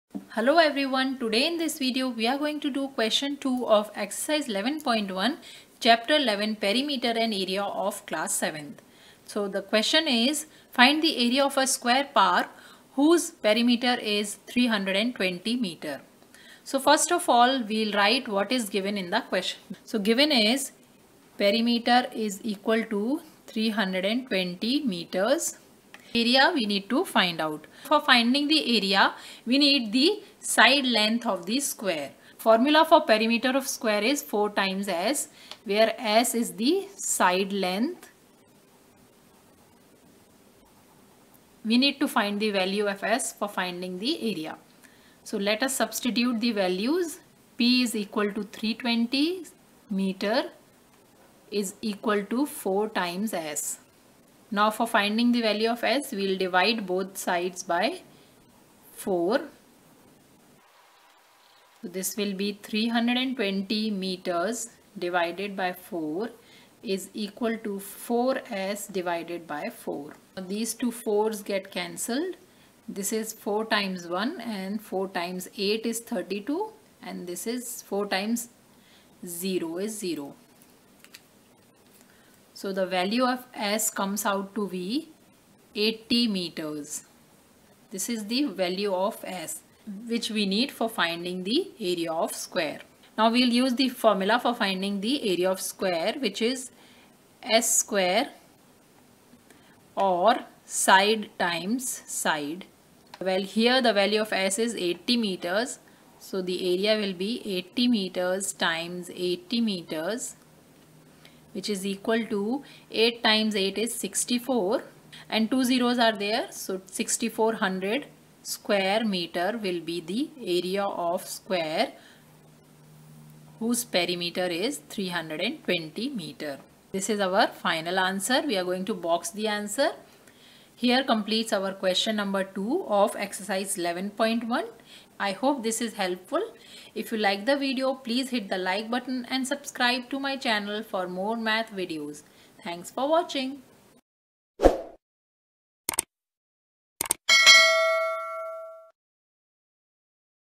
Find the Area of Square when perimeter is Given / Class 7 Math Ex-11.1 Q2 / NCERT math class 7
This is a step by step video tutorial on Find the Area of Square when perimeter is Given / Class 7 Math Ex-11.1 Q2 / NCERT math  class 7